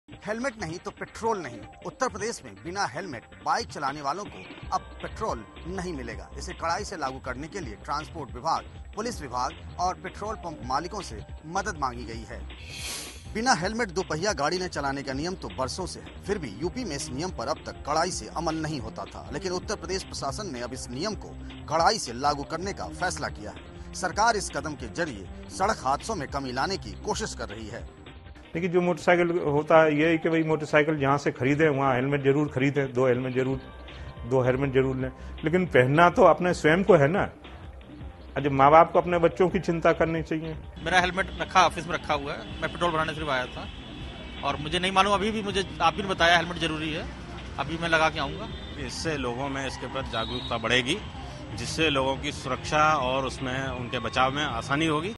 Helmet must for bikers to buy petrol in UP
Two-wheeler riders without helmets will soon be denied fuel at petrol pumps in Uttar Pradesh. State transport minister Raja Mahendra Aridaman Singh issued the order on Tuesday. Singh also made it mandatory for transport vehicles to have a reflector tape on them. "Police and transport department officials should ensure, with the help of petrol pump owners, that petrol is not given to those who are without helmets," Singh said in his order.